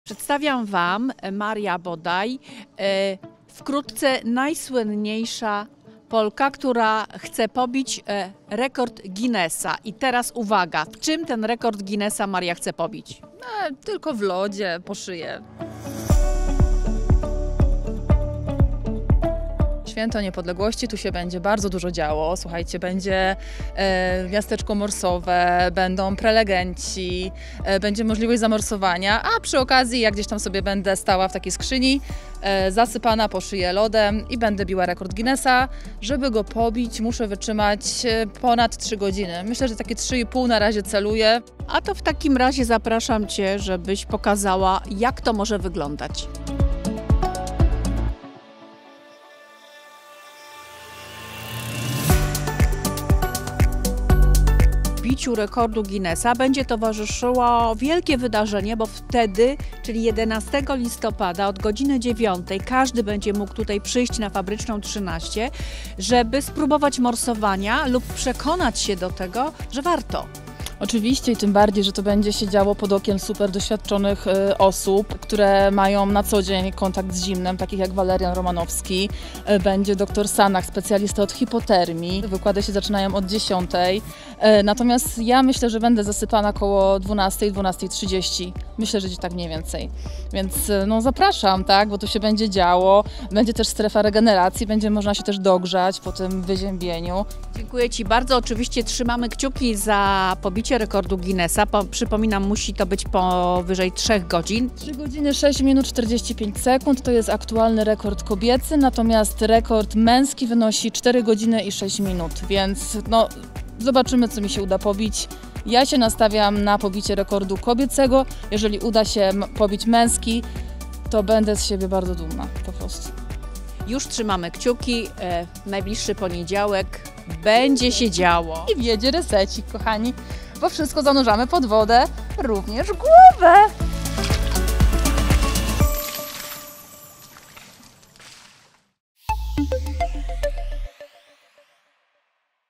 Rekord Guinnessa w morsowaniu
W poniedziałek, 11 listopada Maria Bodaj podejmie próbę pobicia dotychczasowego rekordu Guinnessa w morsowaniu. Aktualny rekord kobiecy wynosi 3 godziny 6 minut 45 sekund. Od godz. 9.00 na ul. Fabrycznej 13 w specjalnym miasteczku morsowym, pod okiem doświadczonych osób każdy będzie mógł spróbować morsowania i przekonać się, że warto to robić.